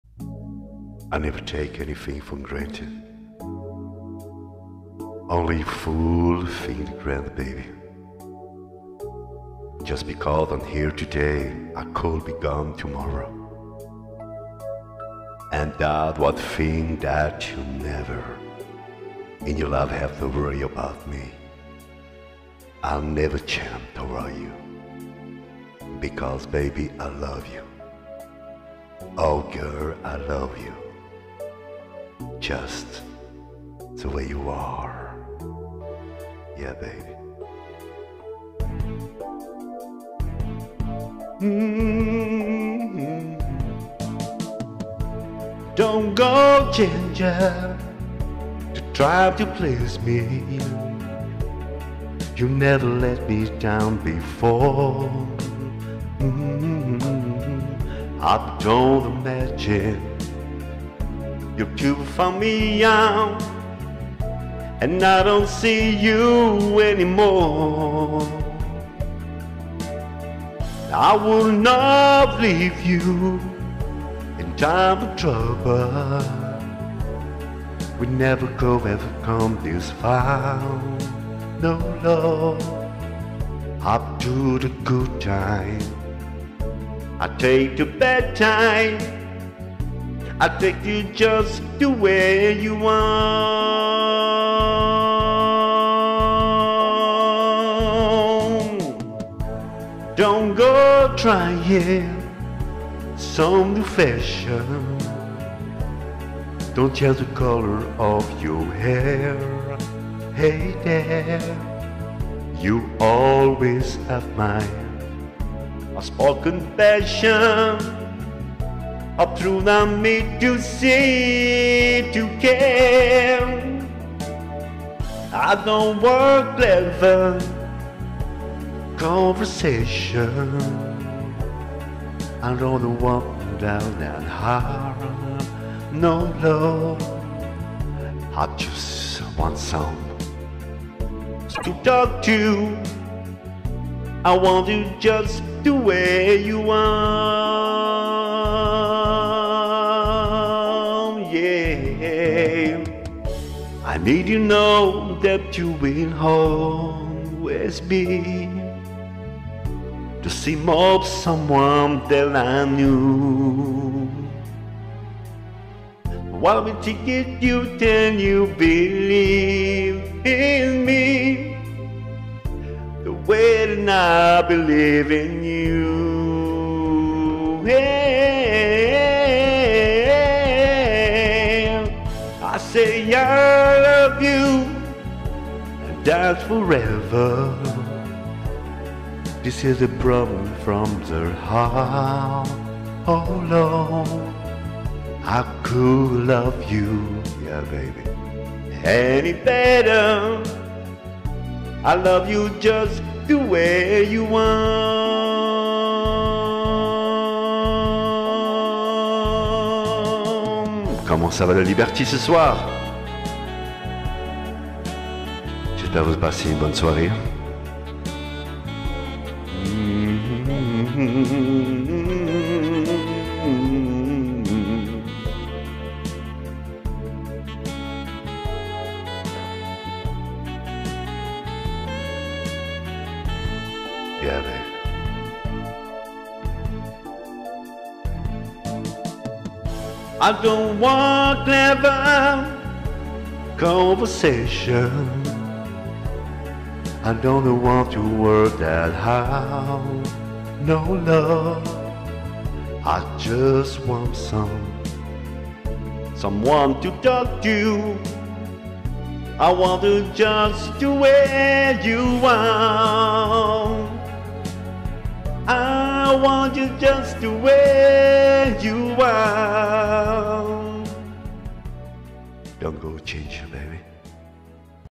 Mikky Mia Singer
Blues, Pop,Salsa, Zouk, Loves songs international variety.
Mikky Miami is a French singer-songwriter whose interpretations combine quality music, a warm personality and a sense of humor. he has a voice that can melt hearts

Liberty Club  Club francophone
Club francophones, rencontre amicale, musique tres varié 70,80  musique de maintenant
club avec ambiance